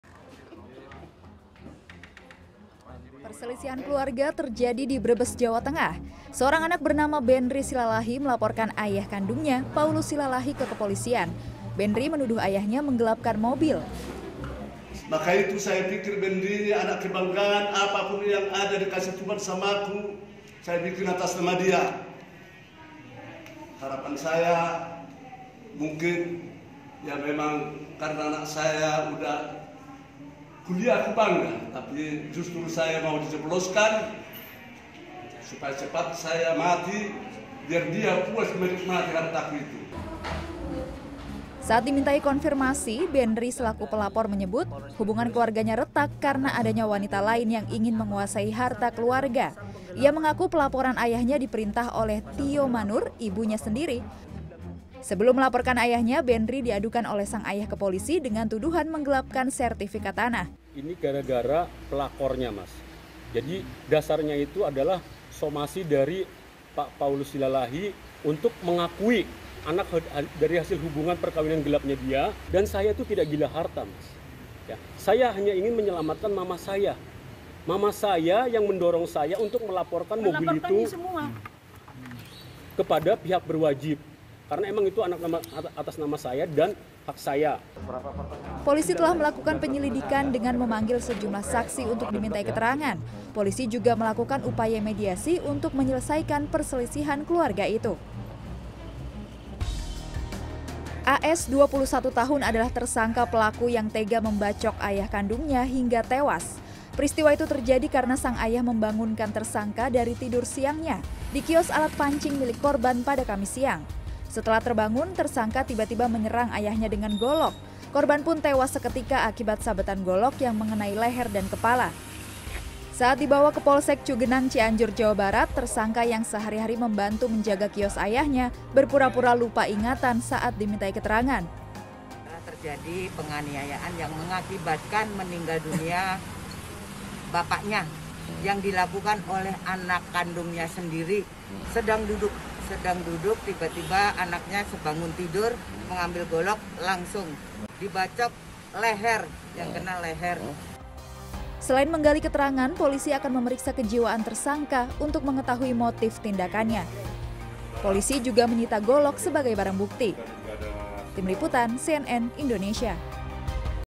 Persoalan Harta, Anak & Ayah Saling Lapor ke Polisi
Perselisihan keluarga terjadi di Brebes, Jawa Tengah. Seorang anak bernama Benry Silalahi melaporkan ayah kandungnya, Paulus Silalahi, ke kepolisian. Benry menuduh ayahnya menggelapkan mobil.

Saat dimintai konfirmasi, Benry selaku pelapor menyebut, hubungan keluarganya retak, karena adanya wanita lain yang ingin menguasai harta keluarga. Ia mengaku, pelaporan ayahnya diperintah oleh Tiomanur ibunya.

Sebelum melaporkan ayahnya, Benry diadukan oleh sang ayah ke polisi, dengan tuduhan menggelapkan sertifikat tanah.